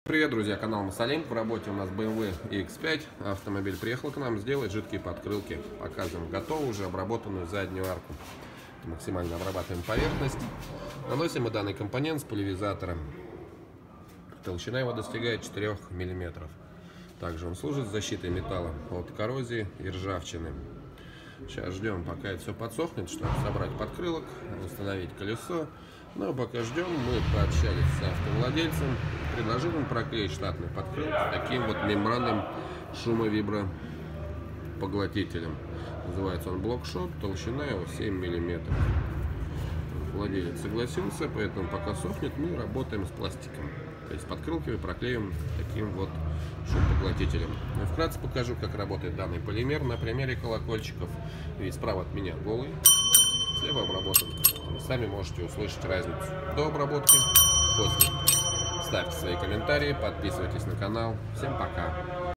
BMW X5, снижаем шум, создаваемый шинами и выбросом песка, грязи и влаги из протектора на локеры
✓ Даже в БМВ Х5 многие не хватает акустического комфорта. Нужно снизить шум от шин, сделать шумоизоляцию арок и локеров также или ещё лучше? Консультация на сайте
Помимо внутрисалонного пакета шумоизоляции, произведена шумоизоляция арок с внешней стороны, в рамках дополнительной шумовиброизоляции и антикоррозийной защиты внутренняя часть арок покрывается составом Промкластер, в основу которого входит антикор в основе битума плюс резиновая крошка, снижаемый шум, создаваемый шинами и выбросом песка, грязи и влаги из протектора на локеры. И сами подкрылки также покрыли шумоблокатором Блокшот толщиной 7 мм. В итоге имеем антикоррозионную и шумоизоляционную защиту, существенное снижения попадания в салон шума от покрышек.